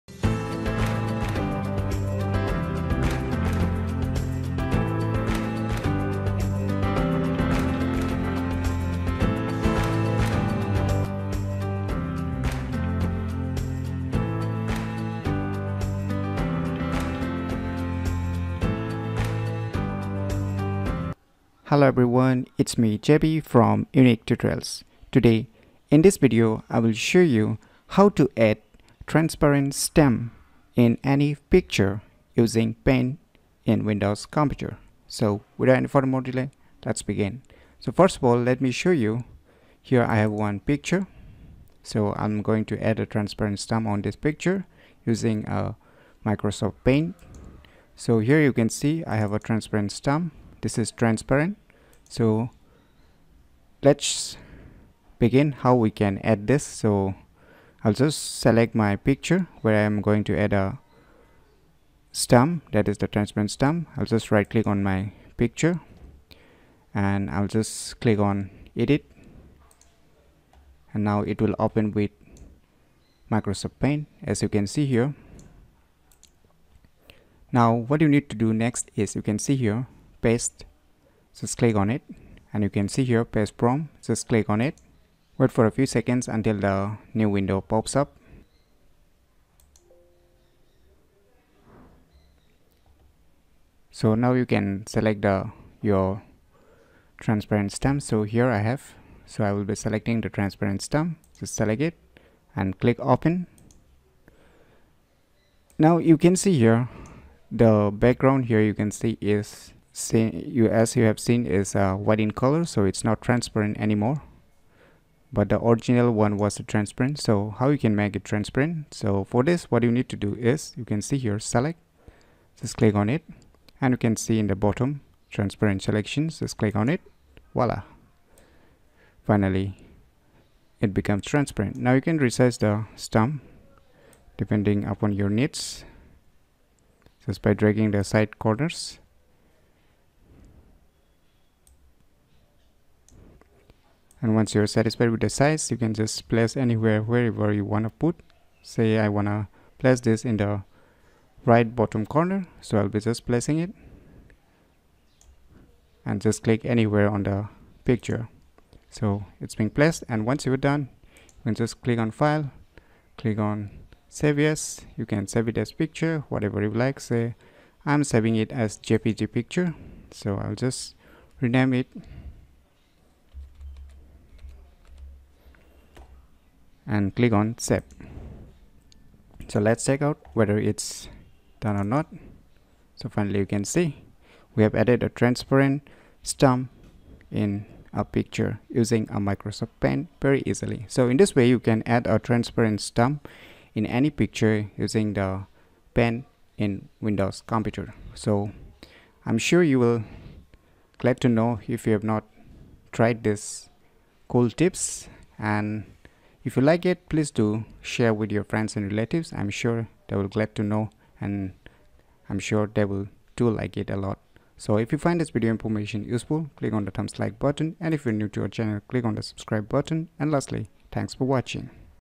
How to add Transparent Stamp in any Picture using Paint in Windows Computer ?
This video tutorial is all about How to add Transparent Stamp in any Picture using Paint in Windows Computer .